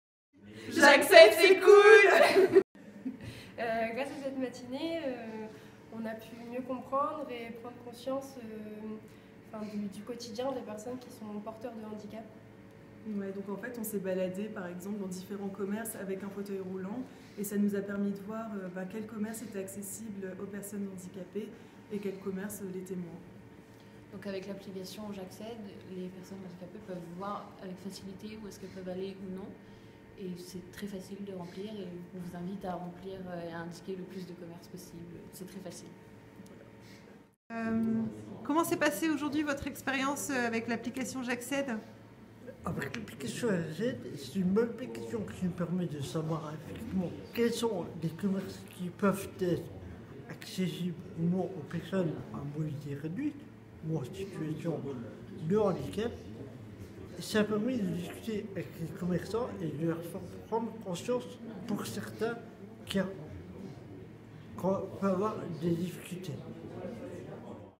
Des bénévoles se mobilisent à Tourcoing pour l'accessibilité
Le 4 avril, la Maison des associations de Tourcoing à invité des bénévoles de l'Université de Lille Métropole pour recenser l'accessibilité des lieux avec l'application JACCEDE. Cela permet aux personnes en situation de handicap de trouver les établissements adaptés à leurs besoins et leurs envies.